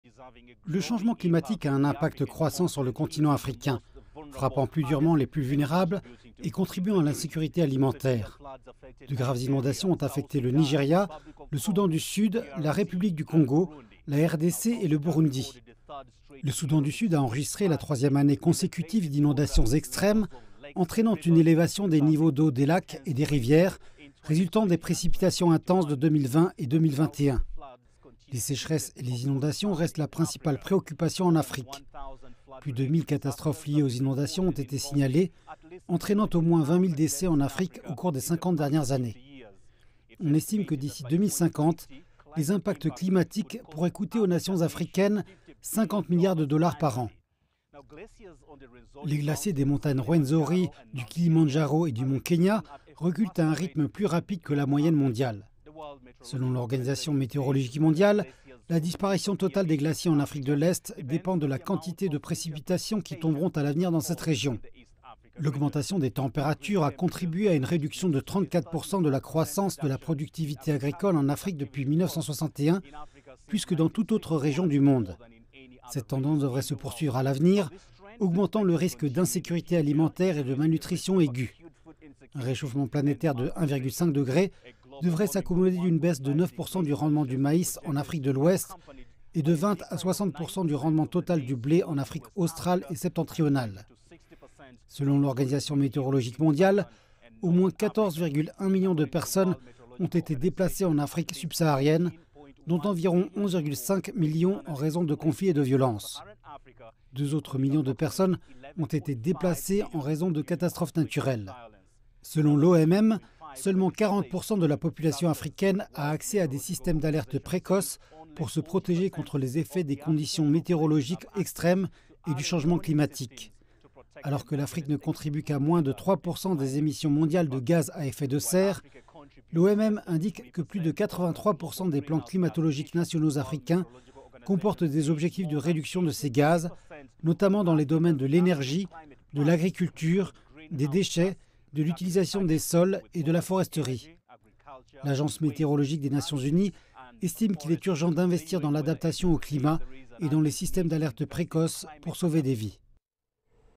Changement climatique : l'Afrique subit des pénuries d'eau, des sécheresses et des inondations
Des pénuries d'eau et des aléas climatiques tels que des sécheresses prolongées et des inondations dévastatrices affectent durement les communautés et les écosystèmes africains. Le régime des pluies est perturbé, les glaciers disparaissent et les principaux lacs et rivières rétrécissent. Selon l'Organisation météorologique mondiale, la demande croissante en eau et des approvisionnements imprévisibles menacent d'aggraver les conflits et les déplacements.